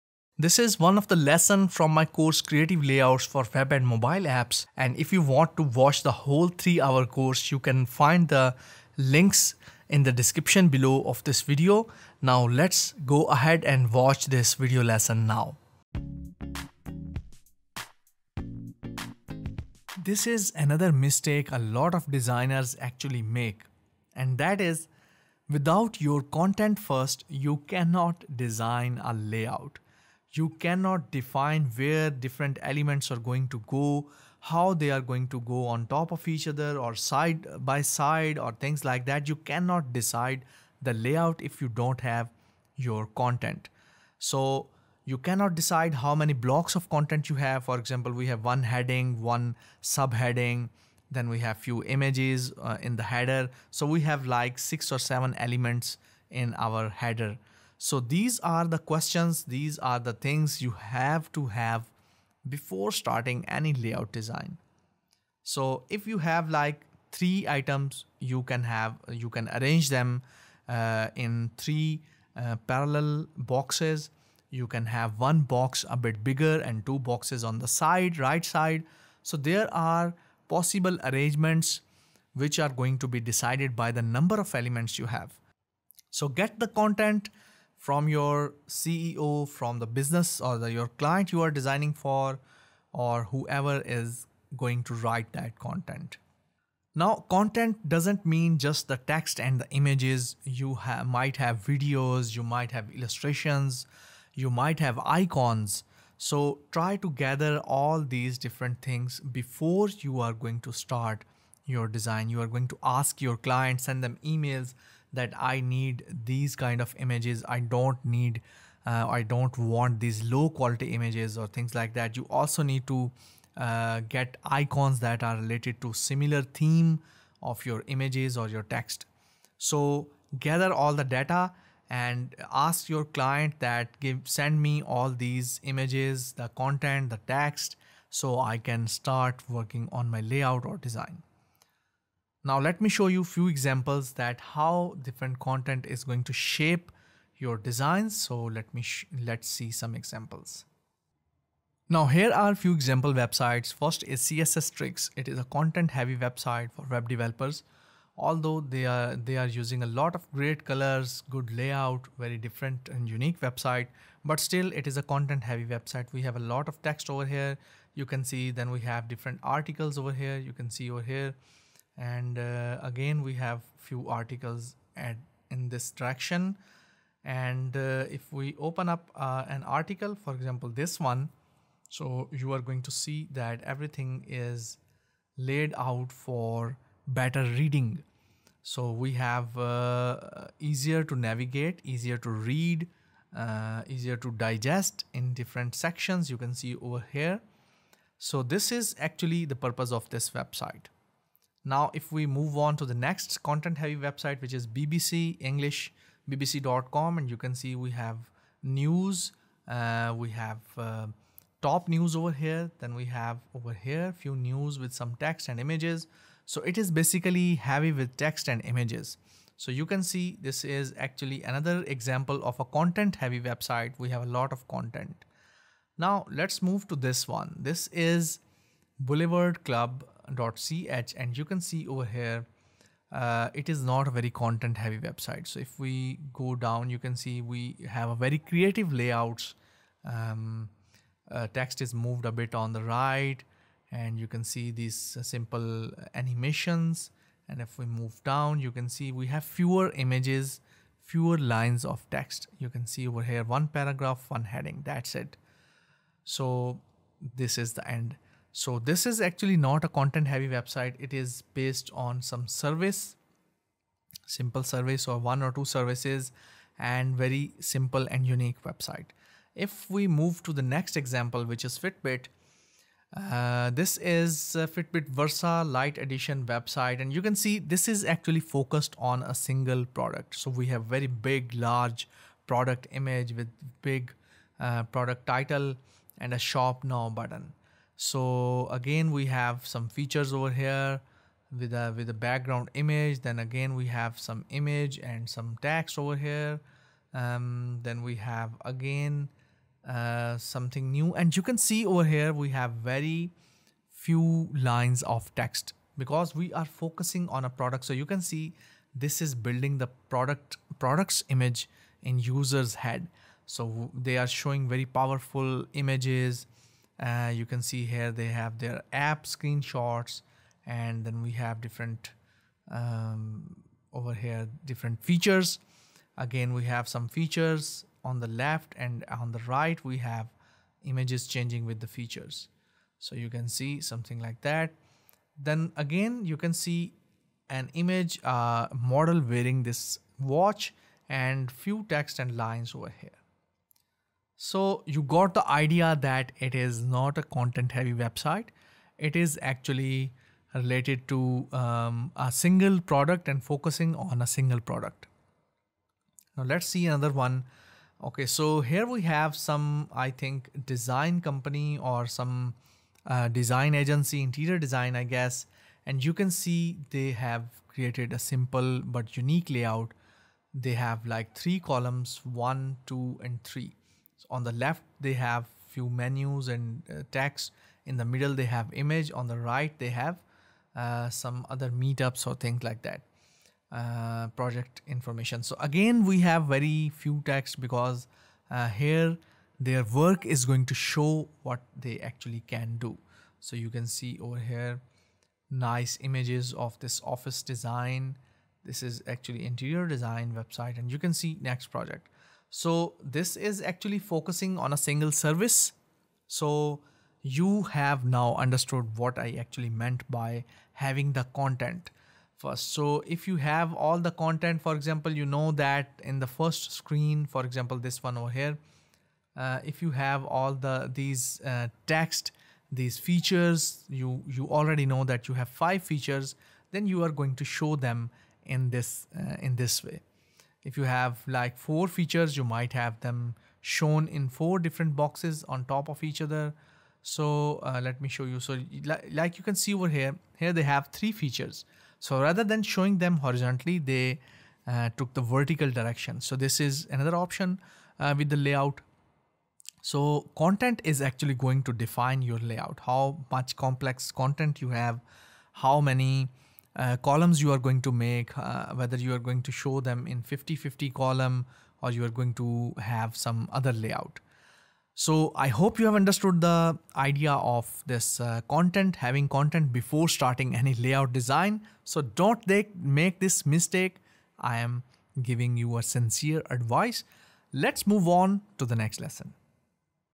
Content will decide the Layout of your Web Design or Mobile Apps
In this Video, you will learn the basics of graphic design and layout design. How content of your website defines the layout for your Web Design or your Mobile App Design.
Watch full course here ►
Creative Layout Design Course (3hr Video Course)

17+ Pro UI UX Design Course bundle for $99 (Biannual)